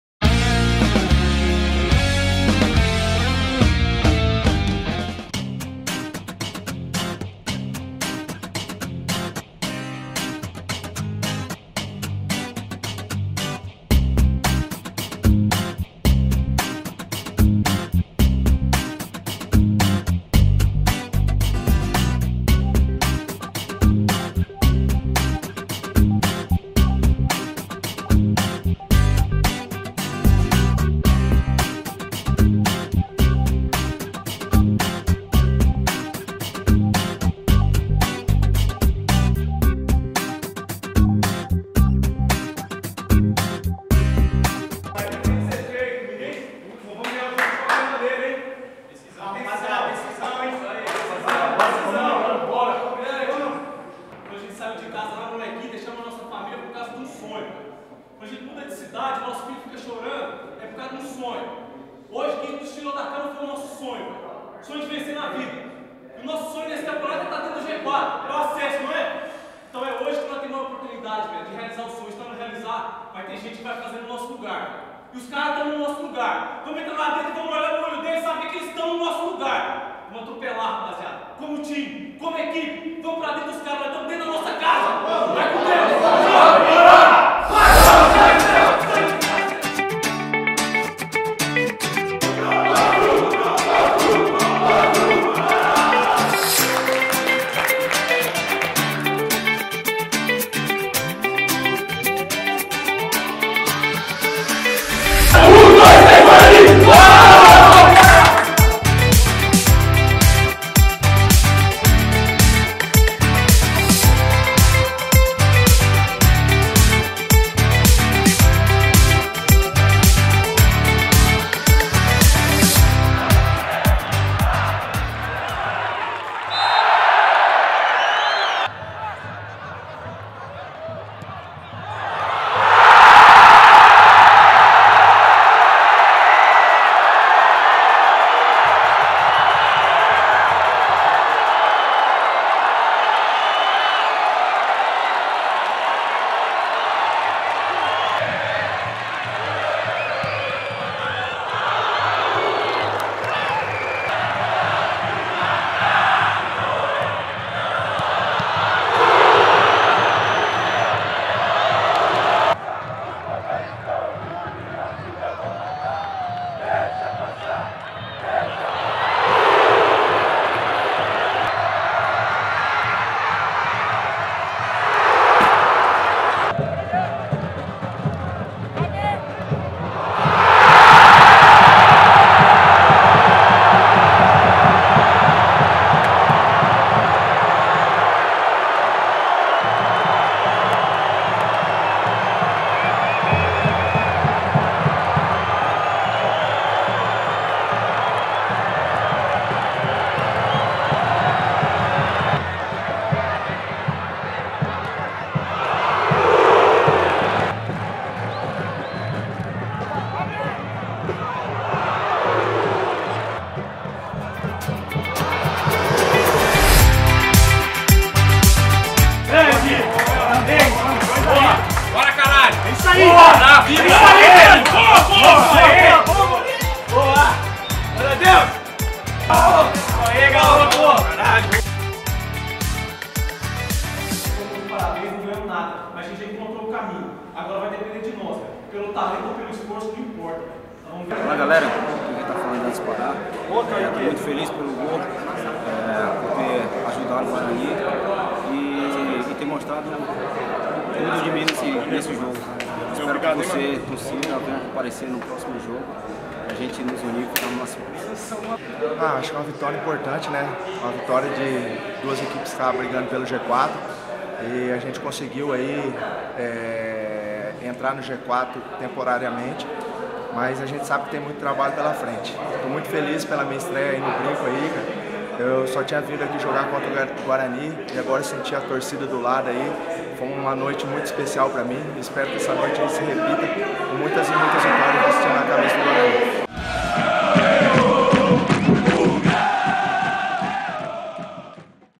BASTIDORES | Guarani 2 x 1 Coritiba
Confira os bastidores da vitória do Bugre em cima do Coritiba pelo placar de 2 a 1. Imagens exclusivas da TV Guarani/IESCAMP